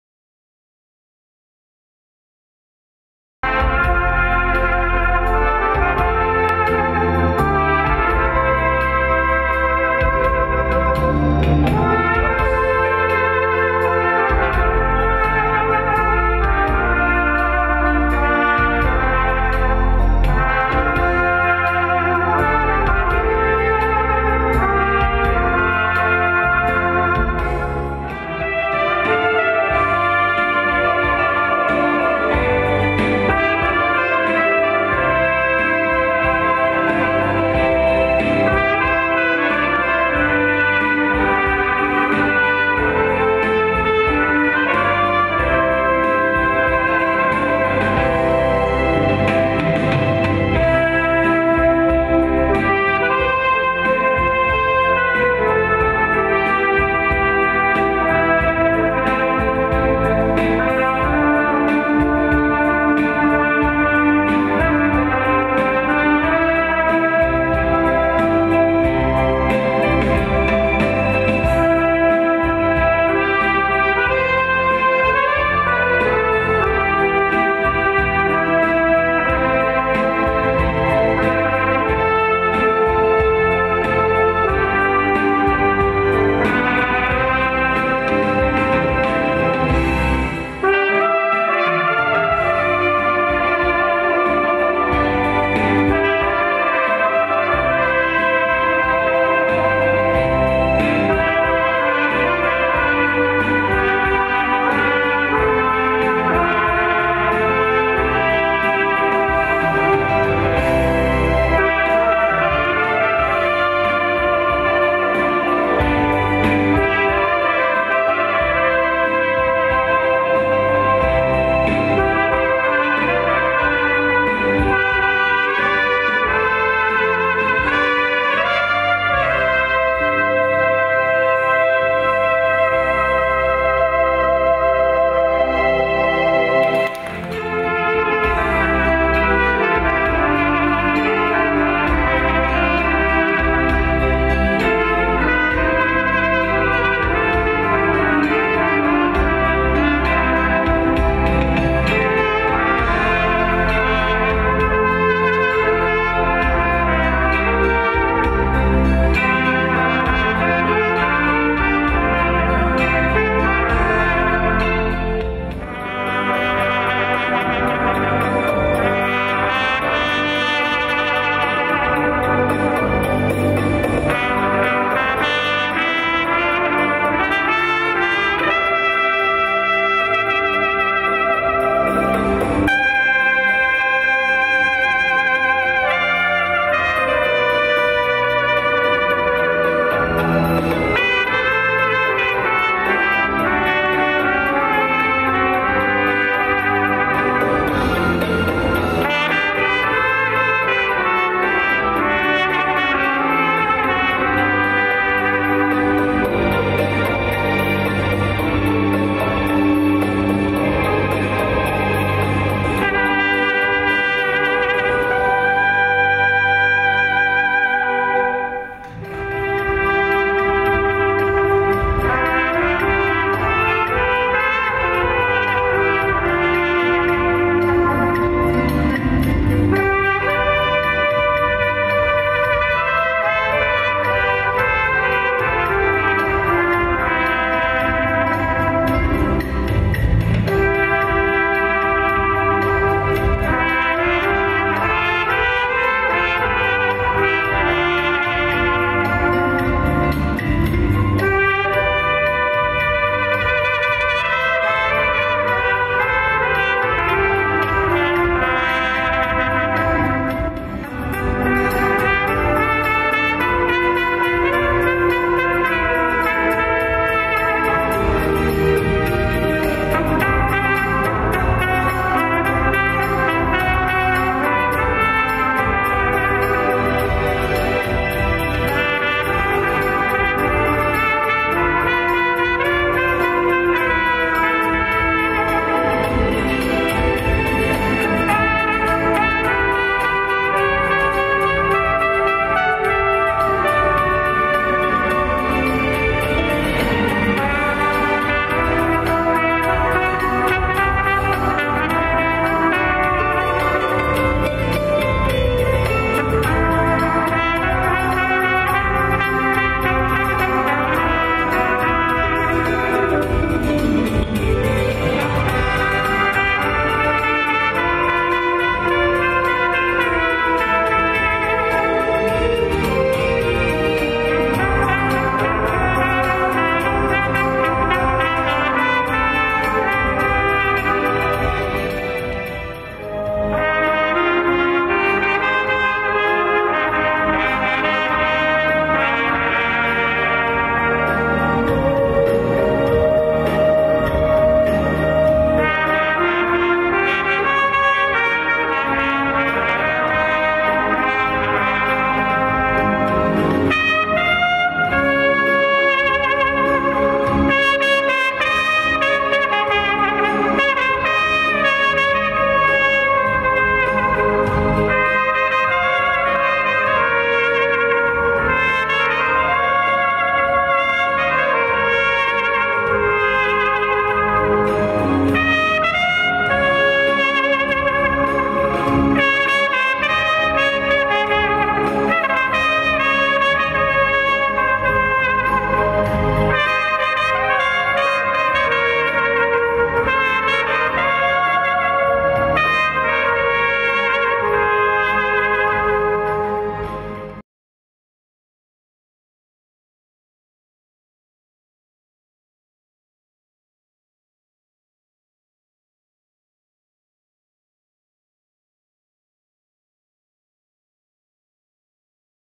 DUO SOFT TRUMPET EN CONCERT images Bernard pastusiak
extraits du concert de DUO SOFT TRUMPET DANS L'EGLISE
DE AMMERSCHWIHR 10 AVRIL 2016